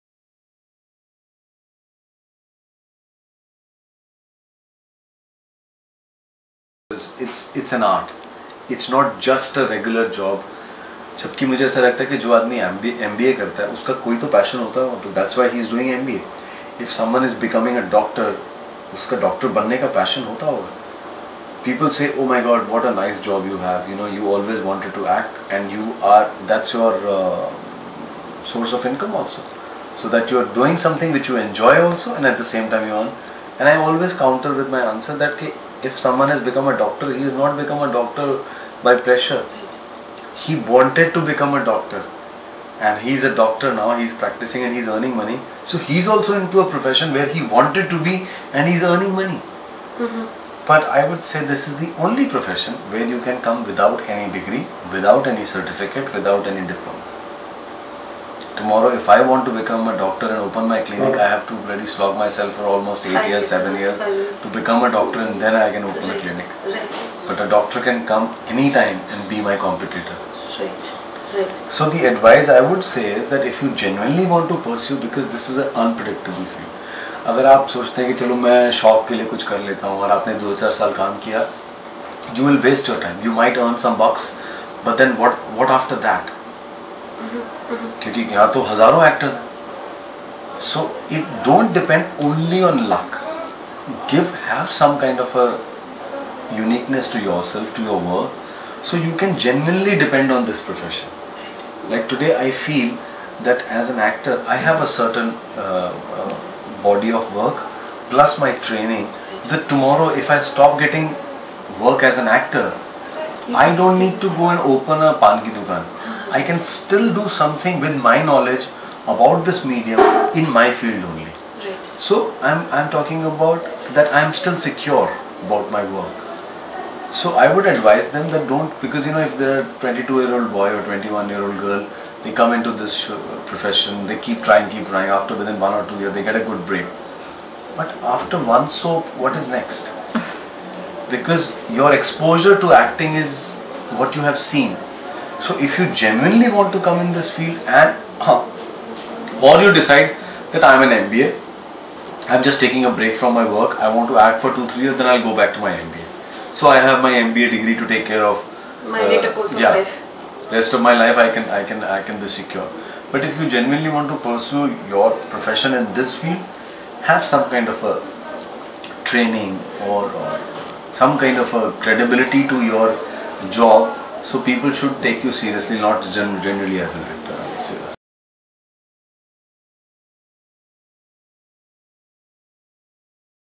Anup Soni-words of wisdom.wmv
Anup Soni advices those seeking a career in acting and about the profession